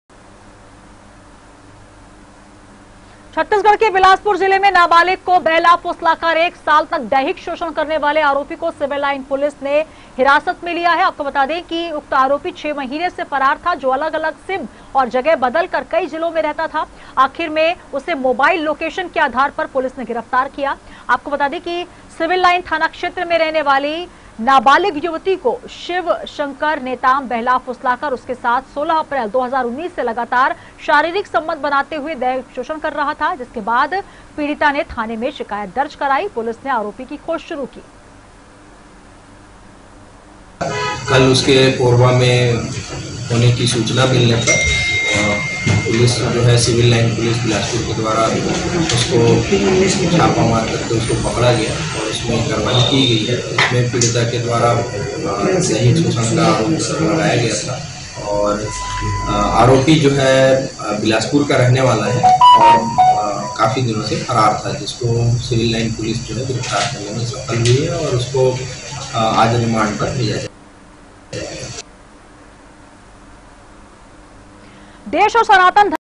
newsworld cg bilaspur दुष्कर्म का आरोपी गिरफ्तार
छत्तीसगढ़ के बिलासपुर जिले में नाबालिग को बहला-फुसलाकर एक साल तक दैहिक शोषण करके वाले आरोपी को सिविल लाइन पुलिस ने गिरफ्तार कर लिया है... उक्त आरोपी 6 महीने से फरार था जो अलग अलग सिम व जगह बदल कर कई जिलों में रहता था... आखरी में उसे मोबाइल लोकेशन के आधार पर पुलिस ने गिरफ्तार कर लिया...अपको बता दे कि... सिविल लाइन थाना क्षेत्र में रहने वाली नाबालिग युवती को शिव शंकर नेताम बहला-फुसलाकर उसके साथ 16 अप्रैल 2019 से लगातार शारीरिक संबंध बनाते हुए दैहिक शोषण करता रहा... इसके बाद जब पीड़िता ने थाने में शिकायत दर्ज कराई तो पुलिस ने आरोपी की खोज शुरू की...इसके बाद टीम गठित कर आरोपी को कोरबा से गिरफ्तार कर लिया गया है...

बाईट- उमेश कश्यप, एएसपी सिटी

दुष्कर्म का आरोपी गिरफ्तार
6 महीने से फरार था आरोपी शंकर नेताम
मोबाइल लोकेशन के आधार पर पुलिस ने पकड़ा
बहला-फुसलाकर लगातार करता था दैहिक शोषण